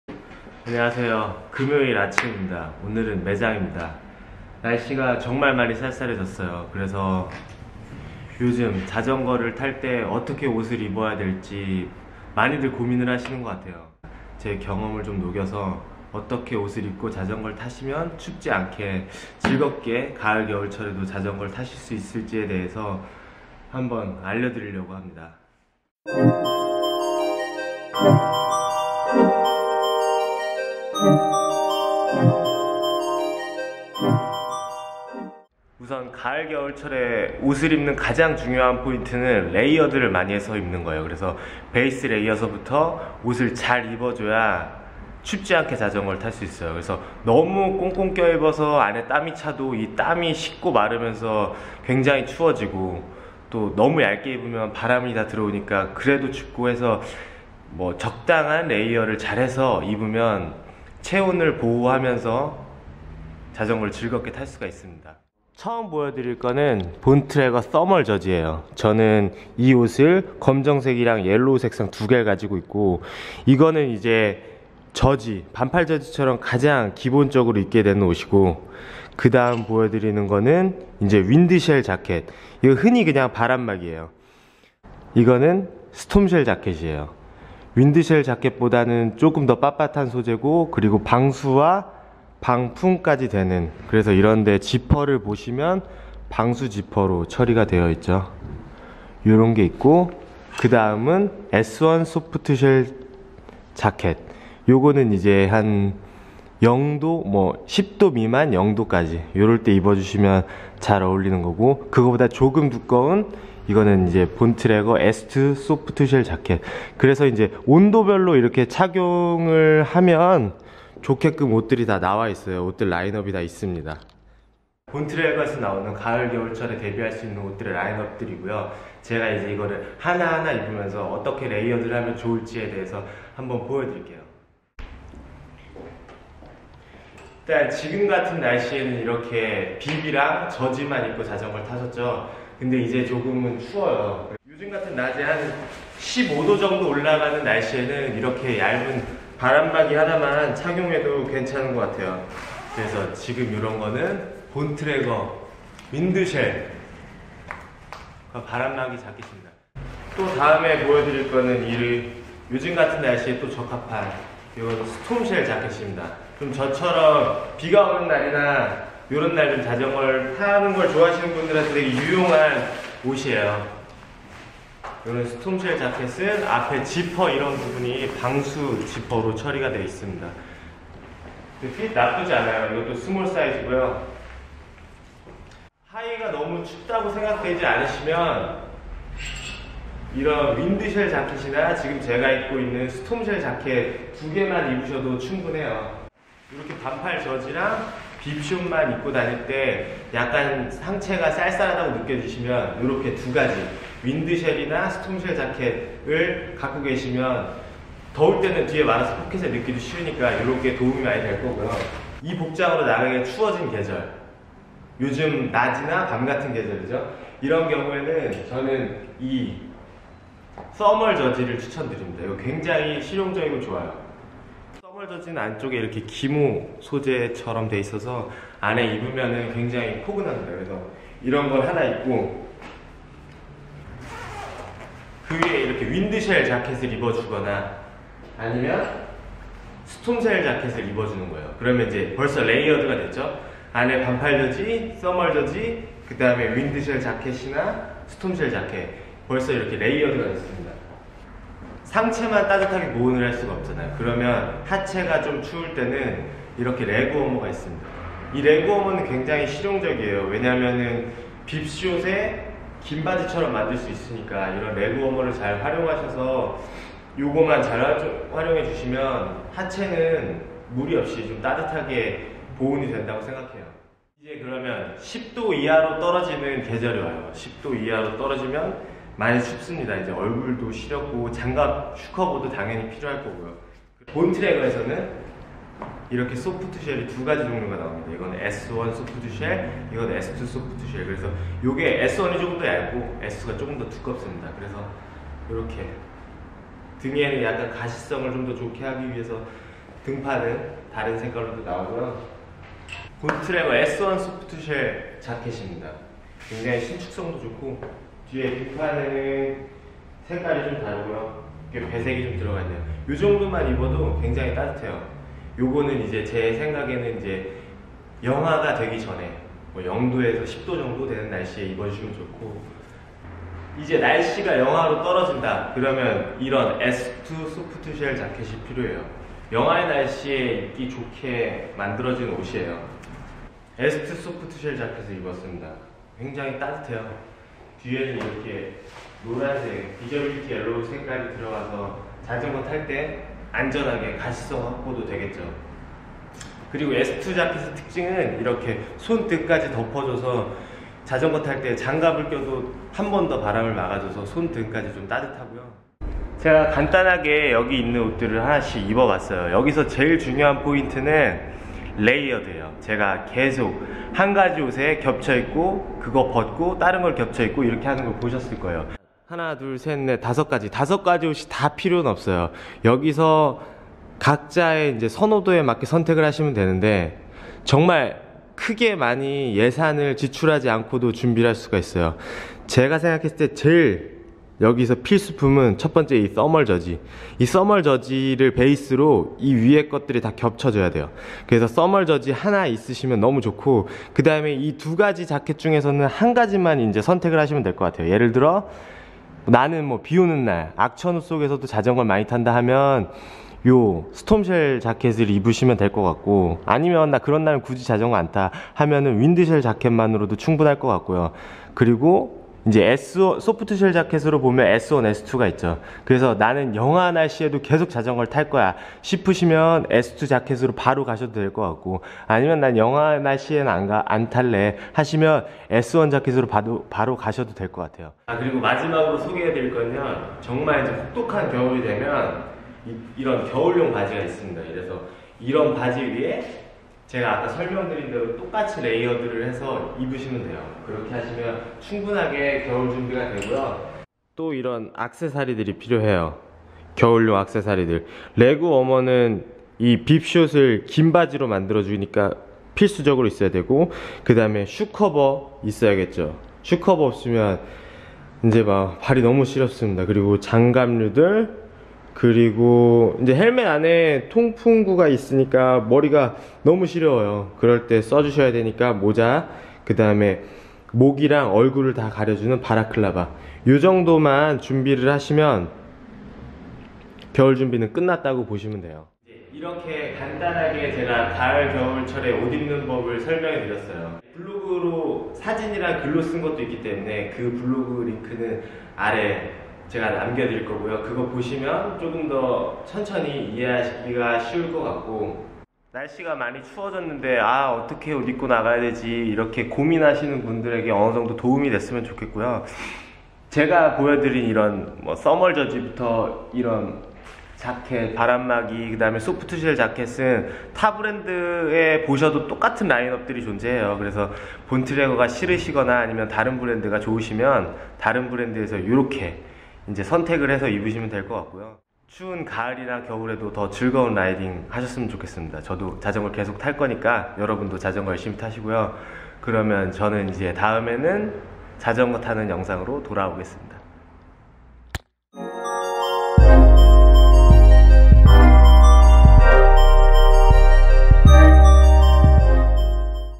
민부리또 | 가을&겨울철 자전거 옷 입는 방법 with 본트레거 ; FALL & WINTER CYCLING WEAR TIPS WITH BONTRAGER
CYCLING REVIEW

안녕하세요 날씨가 많이 추워졌습니다.
그래서! 가을& 겨울철 온도 별 자전거 옷 입는 방법에 대해서
제 실제 경험을 녹여서 영상을 만들어 보았습니다.

추운 겨울에 어떻게 옷을 입고 자전거를 타야할 지 고민하시는 분들께
많은 도움이 되길 바랍니다.

저는 키 179 / 몸무게 67
동영상에서 입은 모든옷은 S 사이즈를 착용했습니다.

사진이나 글로 작성된 포스팅은 아래 블로그 링크를 통해서 확인해주세요.

본트레거 의류 가격에 대한 정보는 아래 링크에서 확인해주세요.

* CAMERA | CANON M6 ;
* LENSE | CANON EF-M 22mm f2 STM;
* MICROPHONE | Rode VideoMicro Compact ;